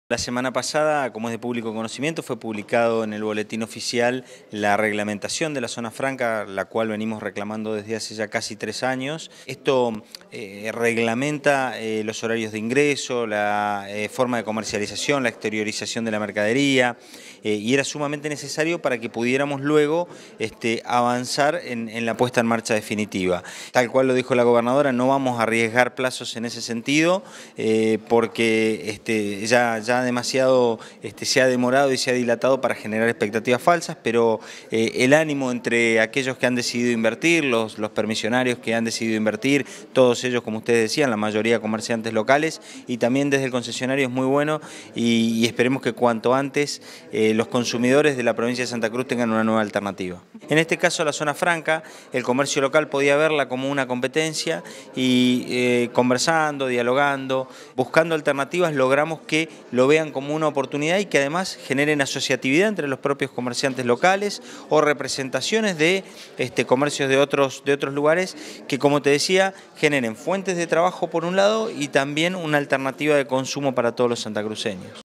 Álvarez: “El ánimo entre los que han decidido invertir es muy bueno”
Así lo afirmó Leonardo Álvarez, Ministro de la Producción, en la reunión que mantuvo ayer la Gobernadora junto a concesionarios y permisionarios de la Zona Franca de Río Gallegos.

Leonardo Álvarez, Ministro de la Producción, fue parte del encuentro que mantuvo la Gobernadora ayer junto a concesionarios y permisionarios de la Zona Franca de Río Gallegos. Álvarez sostuvo: “La semana pasada fue publicado en el boletín oficial la reglamentación de la Zona Franca que venimos reclamando hace casi tres años. Esto reglamenta los horarios de ingreso, la forma de comercialización, la exteriorización de la mercadería; era sumamente necesario para avanzar en la puesta en marcha definitiva”.

Álvarez remarcó que, “como dijo la  Gobernadora; no vamos a arriesgar plazos porque ya demasiado se ha demorado y se ha dilatado para generar expectativas falsas pero el ánimo entre los que han decidido invertir, en su mayoría comerciantes locales, es muy bueno”. El Ministro aseveró en relación a ello: “Esperemos que cuanto antes los consumidores de la provincia de Santa Cruz tengan una nueva alternativa”.

En tanto, destacó: “Me parece que la impronta que la Gobernadora nos ha marcado en estos tres años y medio es que aquellas cosas que puedan ser una amenaza hay que convertirlas en una oportunidad o fortaleza para los santacruceños”.

En este caso, continuó el Ministro, “la Zona Franca podía verse como una competencia para el comercio local pero conversando y buscando alternativas logramos que lo vean como una oportunidad y que, además, generen asociatividad entre los propios comerciantes locales, que generen fuentes de trabajo por un lado y también una alternativa de consumo”.